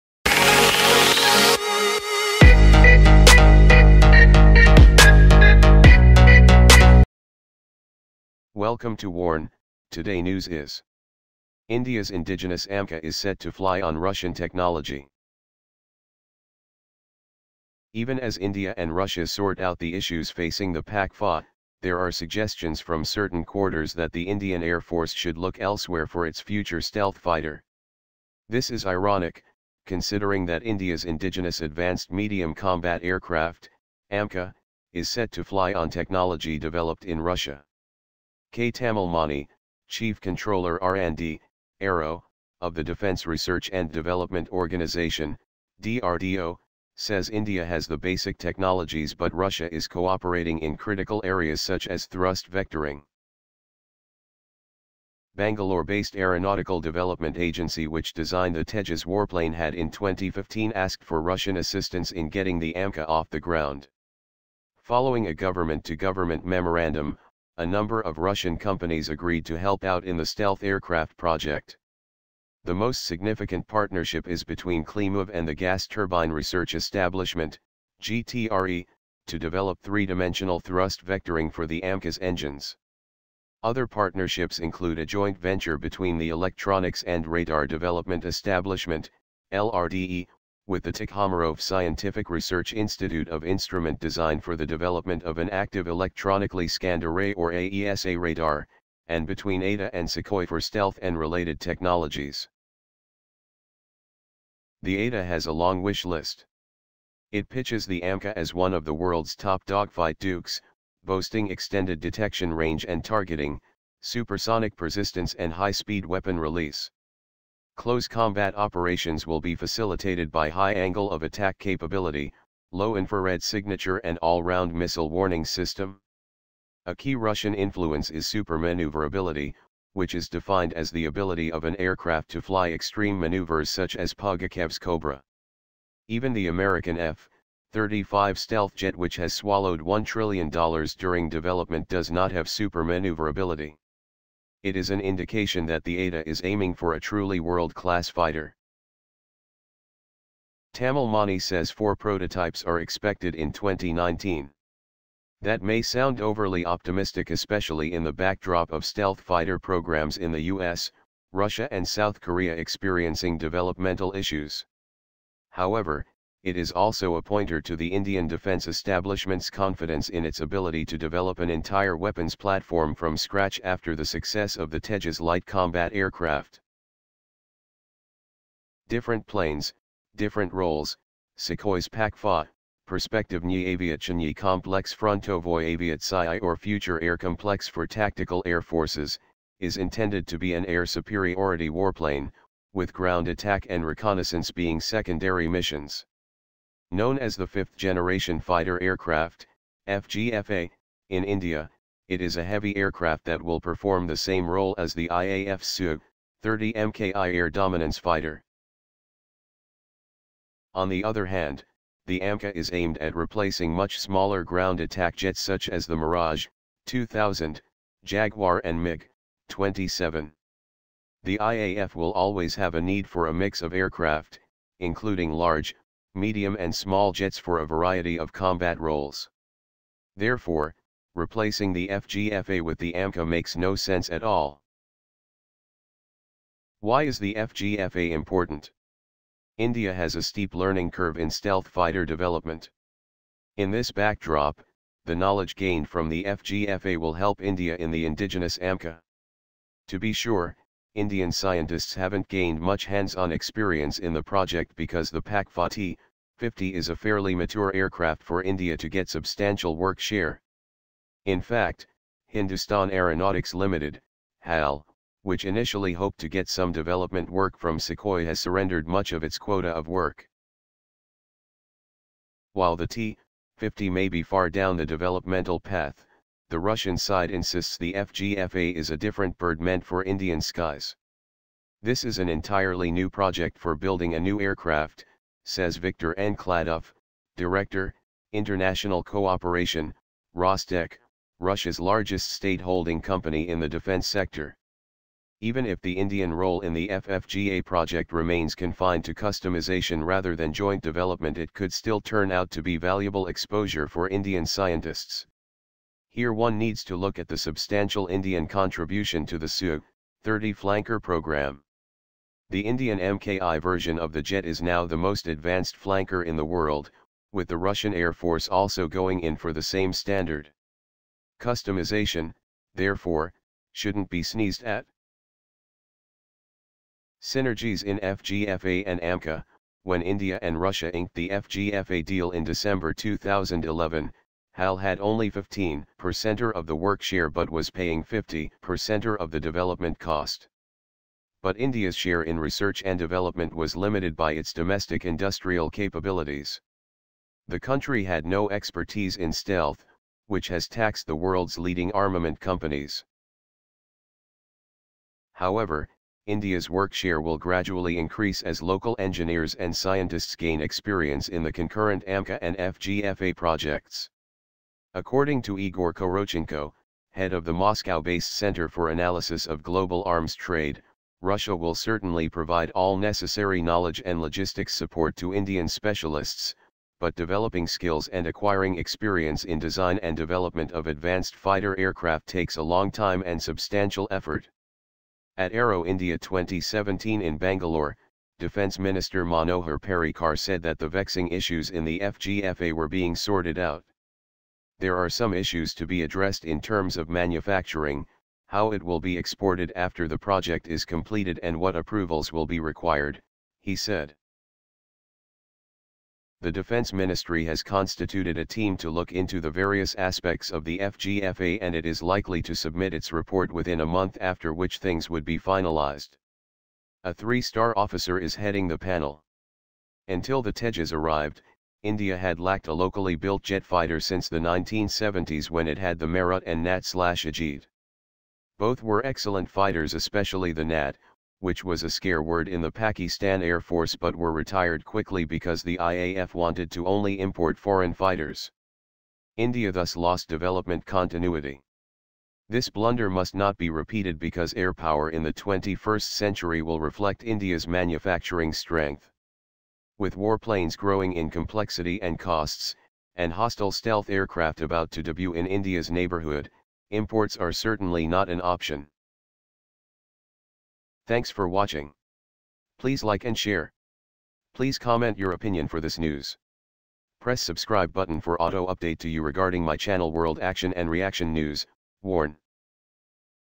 India’s indigenous AMCA is set to fly on Russian technology
Even as India and Russia sort out the issues facing the PAK-FA, there are suggestions from certain quarters that the Indian Air Force should look elsewhere for its future stealth fighter. This is ironic, considering that India’s indigenous Advanced Medium Combat Aircraft (AMCA) is set to fly on technology developed in Russia.
K. Tamilmani, Chief Controller R&D (Aero) of the Defence Research & Development Organisation (DRDO), says India has the basic technologies but Russia is cooperating in critical areas such as thrust vectoring.

Bangalore-based Aeronautical Development Agency – which designed the Tejas warplane – had in 2015 asked for Russian assistance in getting the AMCA off the ground. Following a government to government memorandum, a number of Russian companies agreed to help out in the stealth aircraft project. The most significant partnership is between Klimov and the Gas Turbine Research Establishment (GTRE) to develop three-dimensional thrust vectoring for the AMCA’s engines.
Other partnerships include a joint venture between the Electronics & Radar Development Establishment (LRDE) with the Tikhomirov Scientific Research Institute of Instrument Design for the development of an active electronically scanned array or AESA radar, and between ADA and Sukhoi for stealth and related technologies.

The ADA has a long wishlist. It pitches the AMCA as one of the world’s top dogfight dukes, boasting “extended detection range and targeting, supersonic persistence and high speed weapon release”. Close-combat operations will be facilitated by “high angle of attack capability, low infrared signature and all round missile warning system.”
A key Russian influence is supermanoeuvrability, which is defined as the ability of an aircraft to fly extreme manoeuvres such as Pugachev’s Cobra. Even the American F-35 stealth jet – which has swallowed $1 trillion during development – does not have supermanoeuvrability. It is an indication that the ADA is aiming for a truly world class fighter.

Tamilmani says “four prototypes are expected in 2019”. That may sound overly optimistic – especially in the backdrop of stealth fighter programmes in the U.S., Russia and South Korea experiencing developmental issues. However, it is also a pointer to the Indian defence establishment’s confidence in its ability to develop an entire weapons platform from scratch after the success of the Tejas Light Combat Aircraft.

Different planes, different roles ::
Sukhoi’s PAK-FA (Perspektivnyi Aviatsionnyi Kompleks Frontovoi Aviatsyi or Future Air Complex for Tactical Air Forces) is intended to be an air superiority warplane, with ground attack and reconnaissance being secondary missions. Known as the Fifth Generation Fighter Aircraft (FGFA) in India, it is a heavy aircraft that will perform the same role as the IAF’s Su-30MKI “air dominance” fighter.

On the other hand, the AMCA is aimed at replacing much smaller ground attack jets such as the Mirage-2000, Jaguar and Mig-27. The IAF will always have a need for a mix of aircraft, including large, medium and small jets for a variety of combat roles. Therefore, replacing the FGFA with the AMCA makes no sense at all.

Why is the FGFA important?
India has a steep learning curve in stealth fighter development. In this backdrop, the knowledge gained from the FGFA will help India in the indigenous AMCA.
To be sure, Indian scientists haven’t gained much hands-on experience in the project because the PAK-FA T-50 is a fairly mature aircraft for India to get substantial work share. In fact, Hindustan Aeronautics Ltd (HAL) – which initially hoped to get some development work from Sukhoi – has surrendered much of its quota of work.

While the T-50 may be far down the developmental path, the Russian side insists the FGFA is a different bird meant for Indian skies. “This is an entirely new project for building a new aircraft,” says Viktor N. Kladov, Director, International Cooperation, Rostec, Russia’s largest state holding company in the defence sector.

Even if the Indian role in the FFGA project remains confined to customisation – rather than joint development – it could still turn out to be valuable exposure for Indian scientists. Here one needs to look at the substantial Indian contribution to the Su-30 Flanker programme. The Indian MKI version of the jet is now the most advanced Flanker in the world, with the Russian Air Force also going in for the same standard. Customisation, therefore, shouldn’t be sneezed at.

Synergies in FGFA and AMCA ::

When India and Russia inked the FGFA deal in December 2011, HAL had only 15 per cent of the work share but was paying 50 per cent of the development cost. But India’s share in research-and-development was limited by its domestic industrial capabilities. The country had no expertise in stealth, which has taxed the world’s leading armament companies.